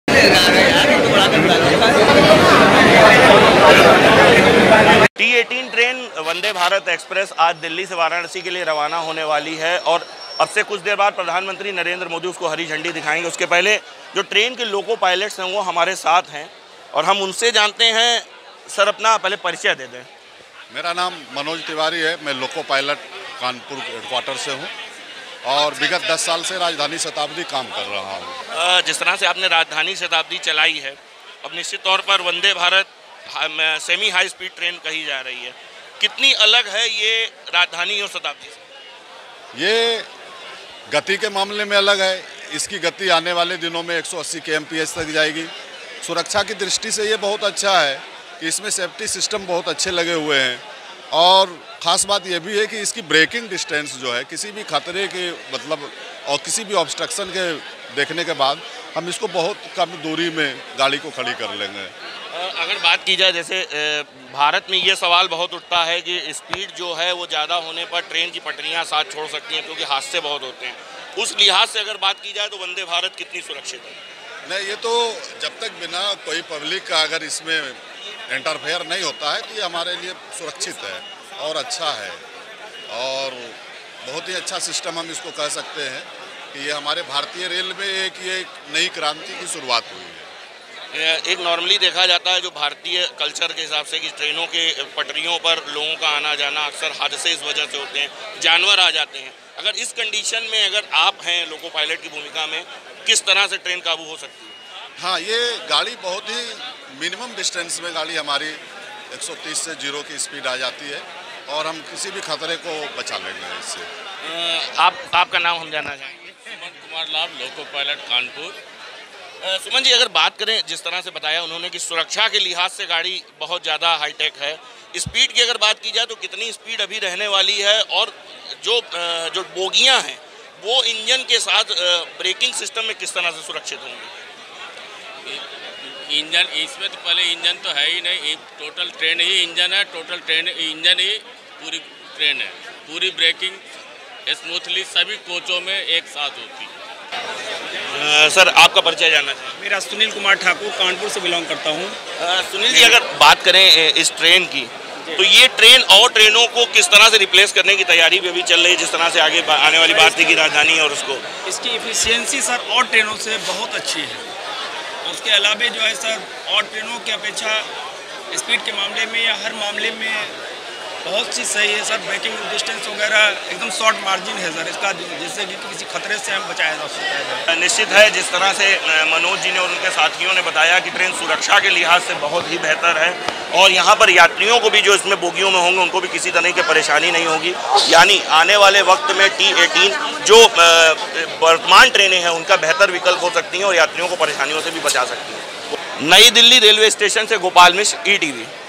आज से ट्रैक पर दौड़ेगी वंदे भारत एक्सप्रेस, 8 घंटे में तय करेगी वाराणसी से दिल्ली का सफर
वाराणसी : देश की पहली सेमी हाईस्पीड ट्रेन वंदे भारत शुक्रवार को दिल्ली से वाराणसी के लिए रवाना होगी. कुछ देर में पीएम मोदी और रेल मंत्री पीयूष गोयल इस ट्रेन को हरी झंडी दिखाकर वाराणसी के लिए रवाना करेंगे. दिल्ली से वाराणसी के बीच का सफर ट्रेन 8 घंटे में पूरा करेगी. इस ट्रेन की स्पीड 130 से 160 किलोमीटर बताई जा रही है, जिसे आगे बढ़ाकर 180 किलोमीटर प्रति घंटा करने की तैयारी है.